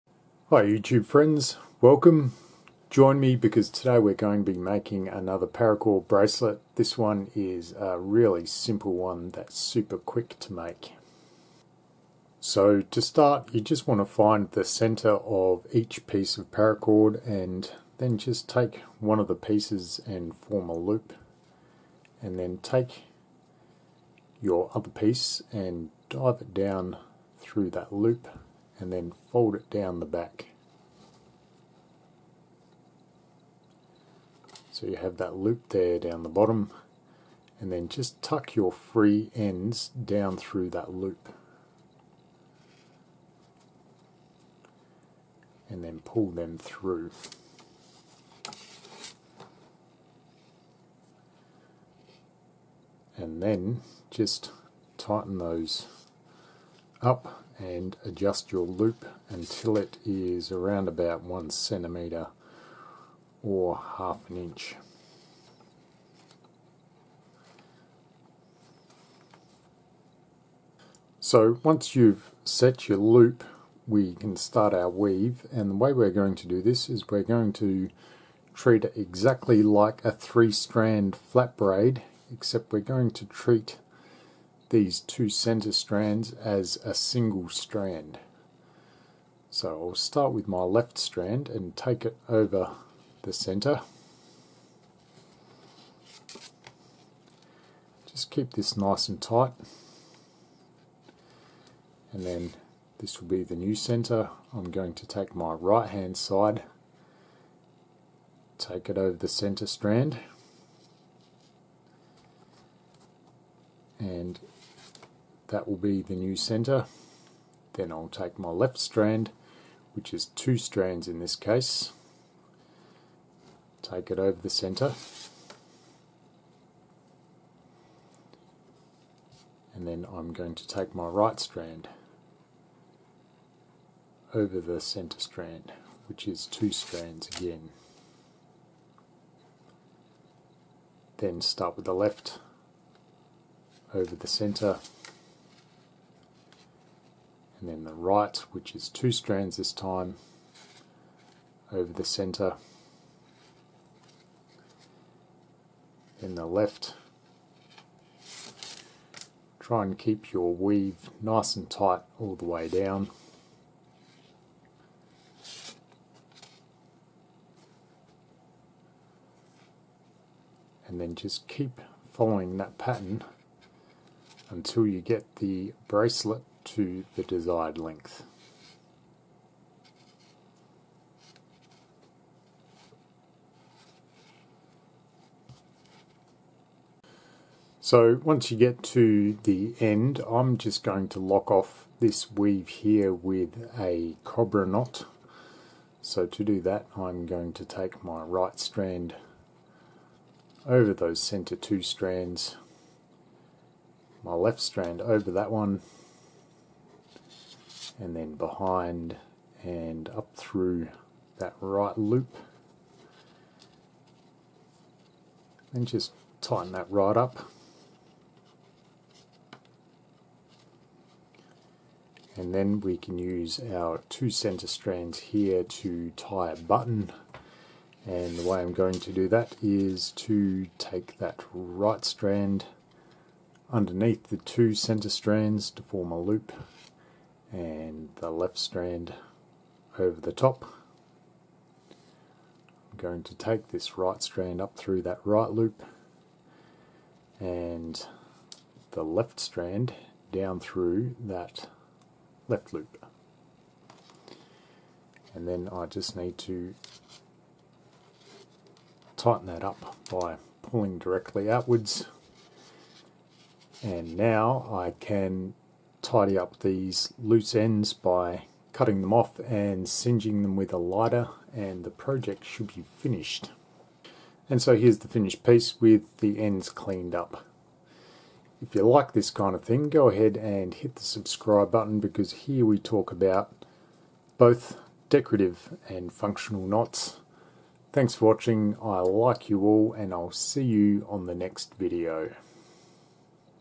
"How You Can Make A Bridged Wanderer Paracord Bracelet"
Instructions for how to tie an easy bridged wanderer paracord survival bracelet with no buckle in this easy step by step DIY video tutorial. This unique 550 cord bracelet is tied with a simple loop and toggle knot closure.

Constructed using a simple three strand braid technique.